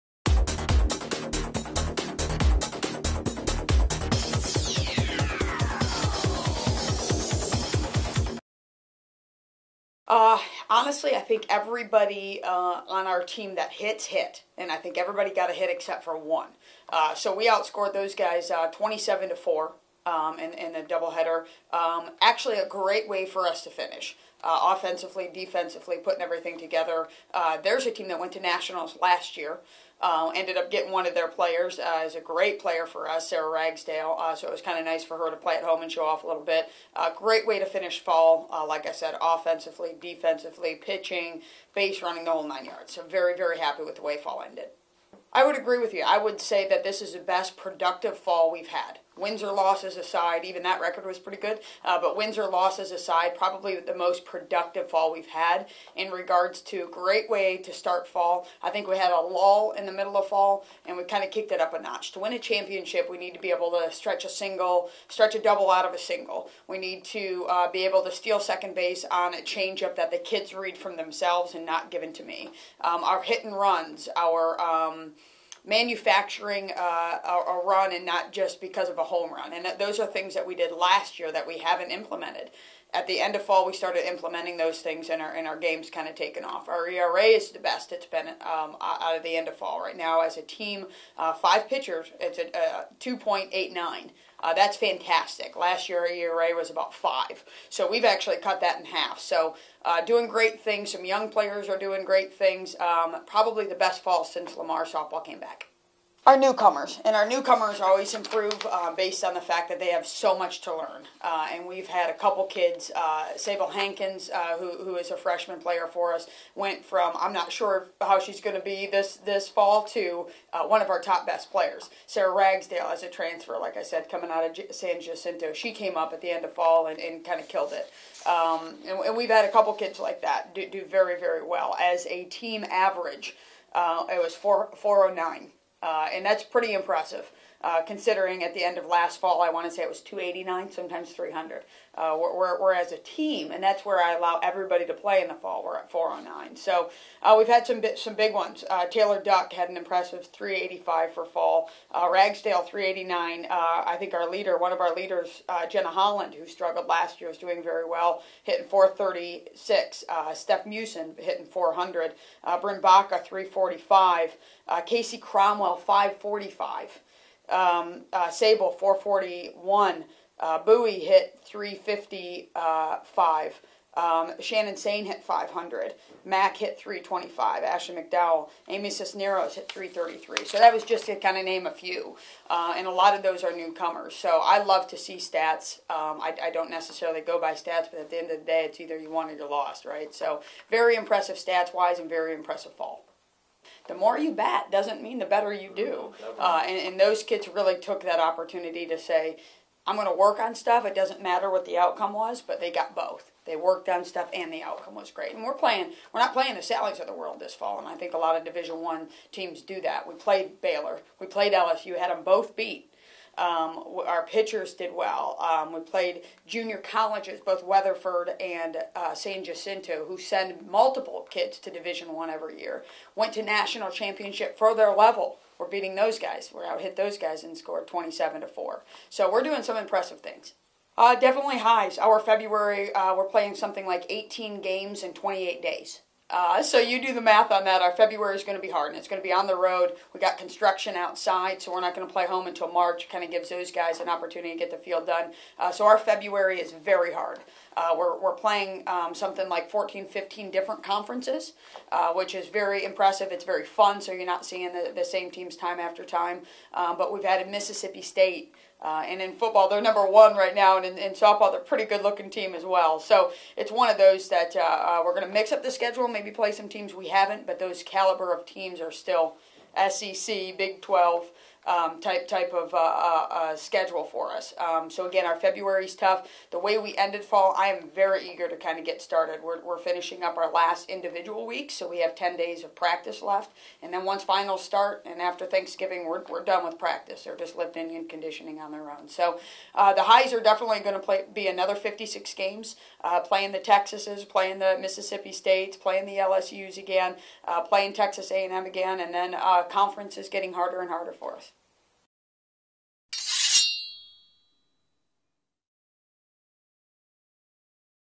FramedUP: Holly Bruder
BEAUMONT: Head Softball Coach Holly Bruder gives an overview of how the best fall season in Lamar Softball history went and looks forward to competing on her brand new softball facility on March 5.